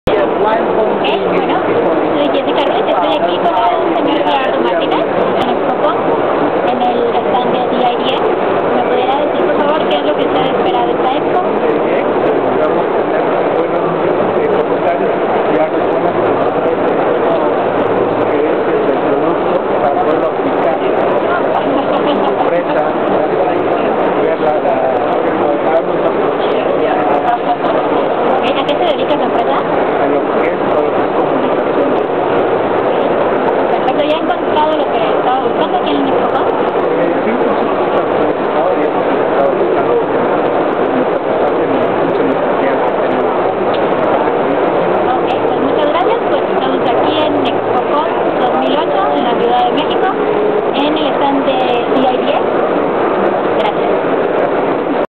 Luis Alberto Moreno of Bas Computers about DIDX at EXPOc
Luis Alberto Moreno (DIDX Partner) of Bas Computers at EXPOcomm shares with you what his company is up to at EXPOcomm in Mexico City and with DIDX!